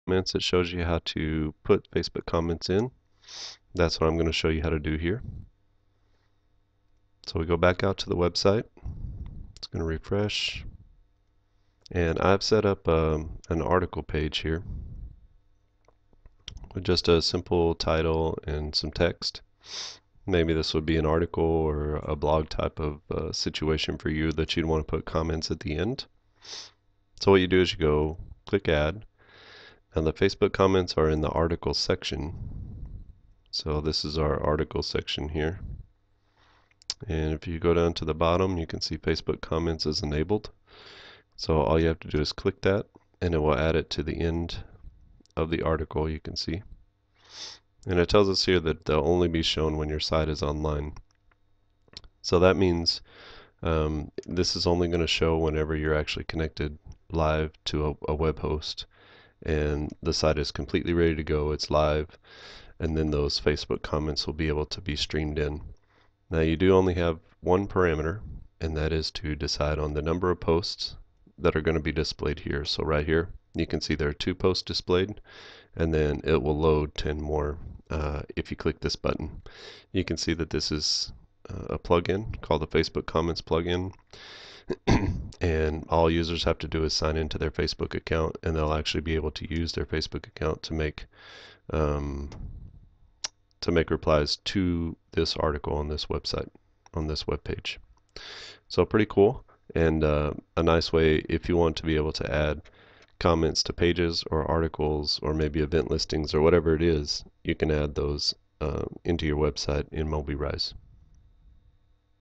Mobirise Facebook Comments | Video 12 of 27
Learn how to use Facebook comments in the Mobirise software. This is one video in a larger project teaching small business owners how to create a website from beginning idea to final publishing online.